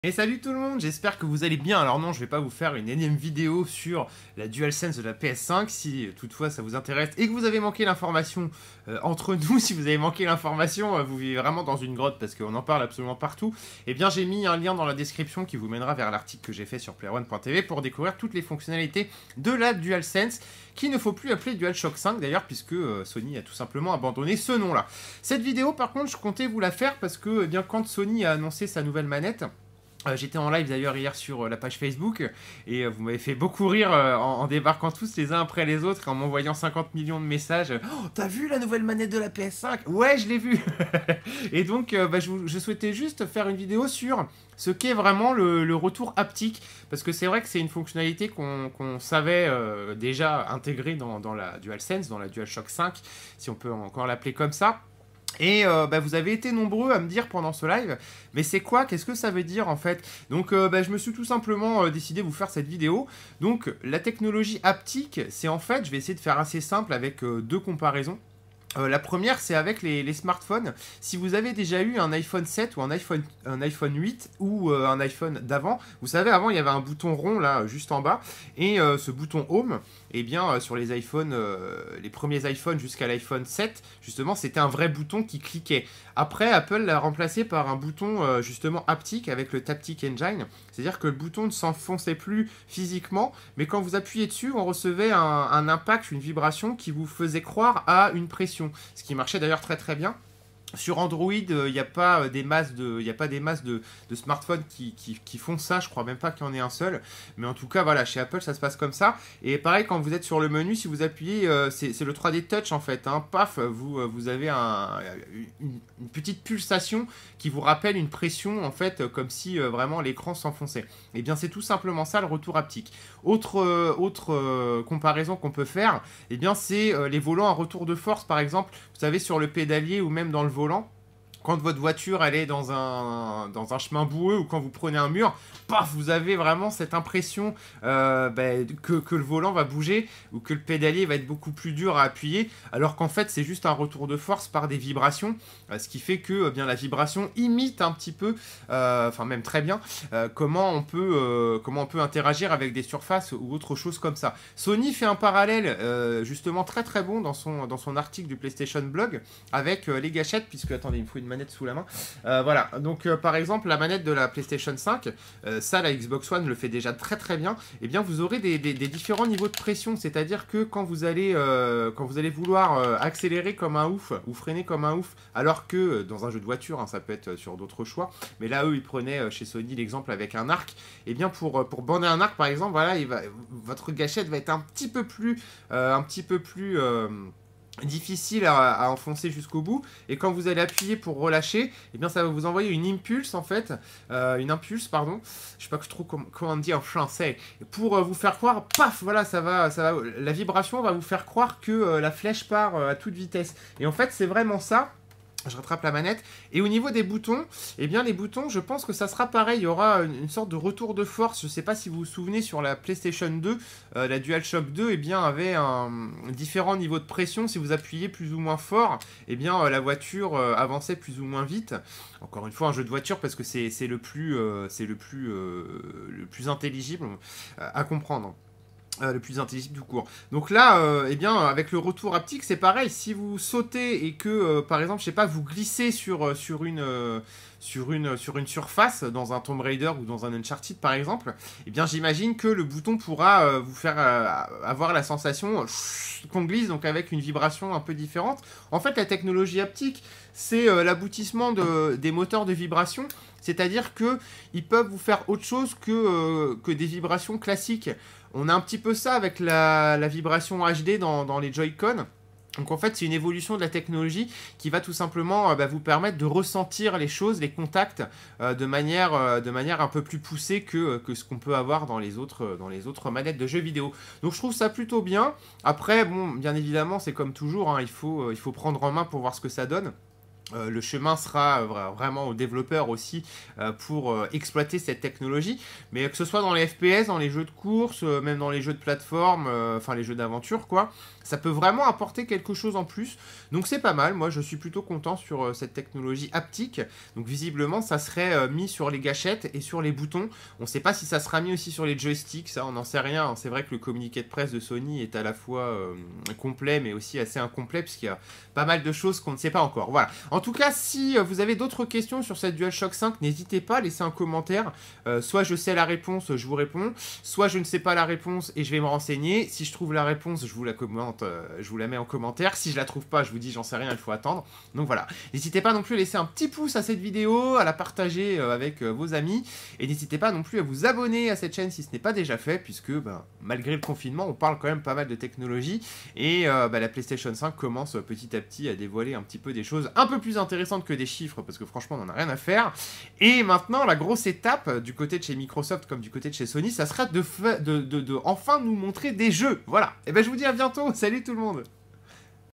PS5 DUALSENSE - CE QU'EST VRAIMENT LE HAPTIQUE DE LA MANETTE ! (EX-DUALSHOCK 5)
➡️ Qu'est-ce que la technologie haptique présente dans la DualSense de la PS5 ? Avec cette nouvelle manette, Sony fait venir une nouveauté sympathique, et je vous explique comment cela fonctionne dans cette vidéo !